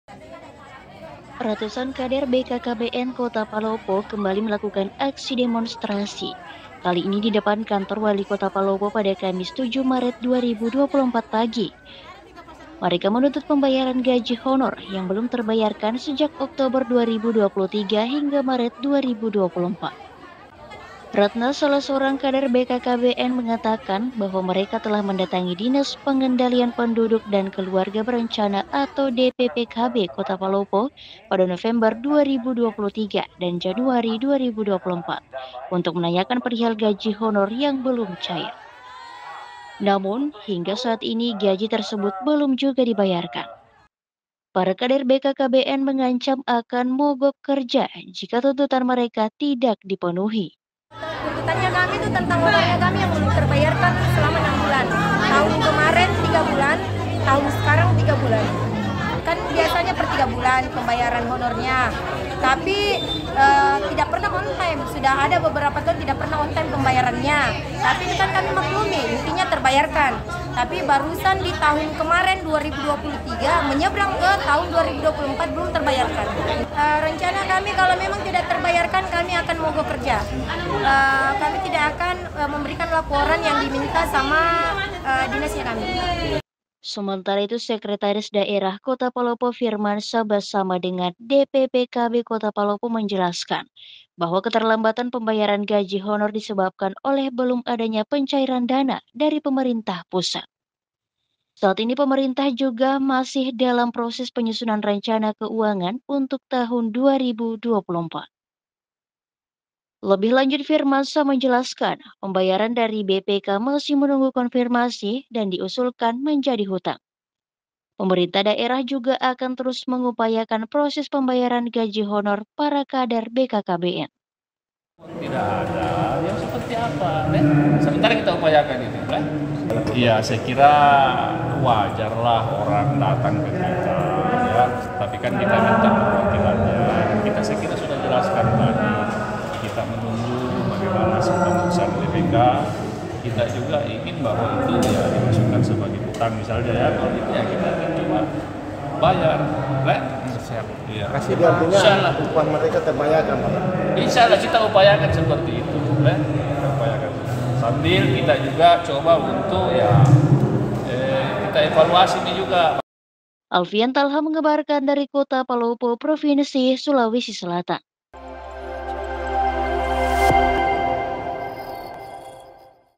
RATUSAN KADER BKKBN PALOPO TUNTUT KEJELASAN HONOR
Sebanyak 500 Kader Keluarga Berencana (KB), Tim Pendamping Keluarga (TPK) dan tenaga Posyandu Kota Palopo mendatangi Kantor Wali Kota Palopo, Sulawesi Selatan (Sulsel). Kedatangan kader tersebut untuk menuntut upah yang belum dibayarkan selama 6 bulan.